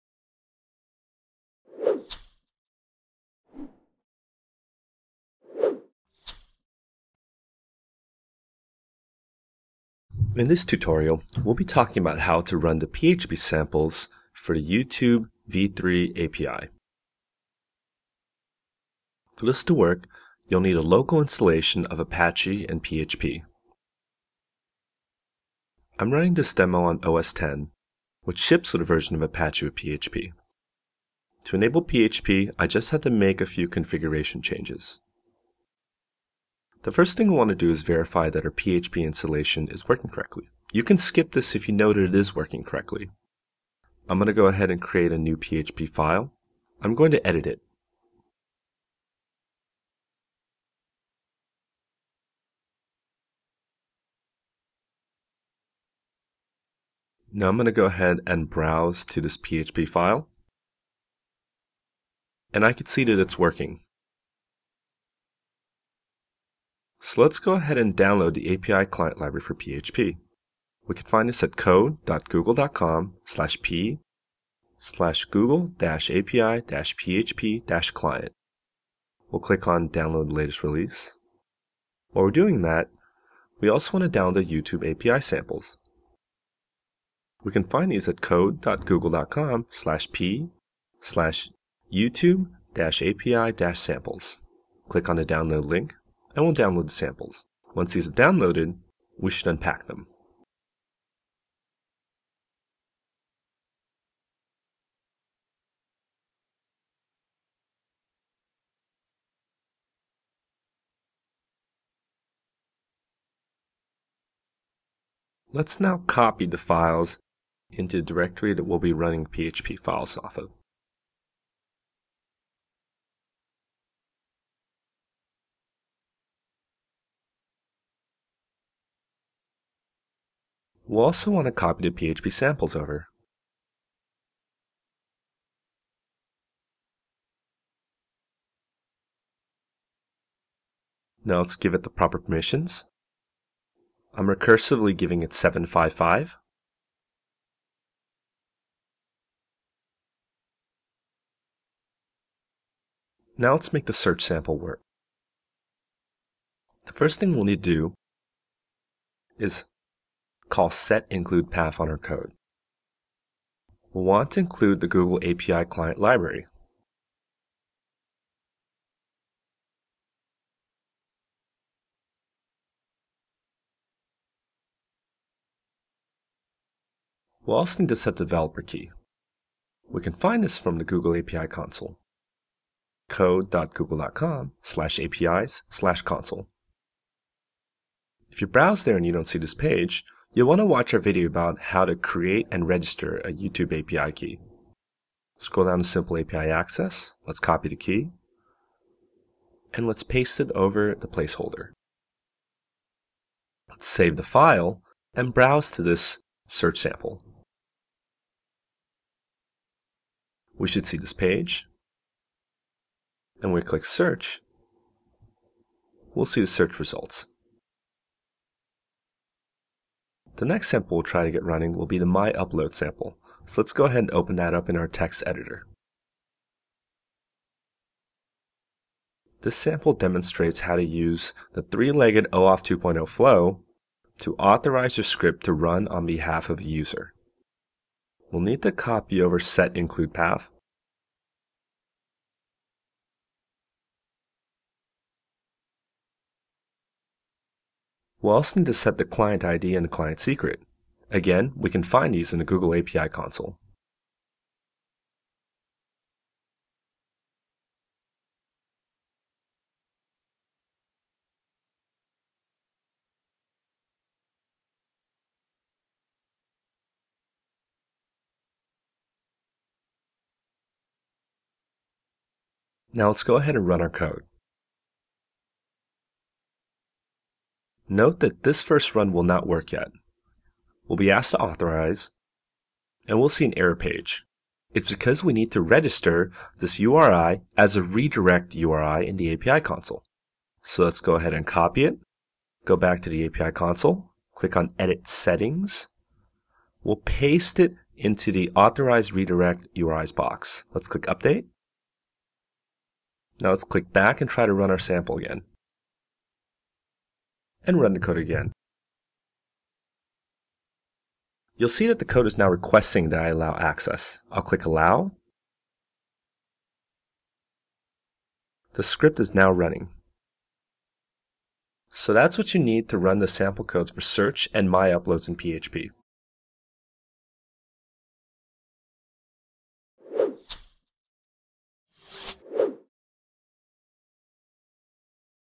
Running the YouTube API PHP code samples
This tutorial demonstrates how to download and run the YouTube API PHP code samples. You'll need a working PHP installation.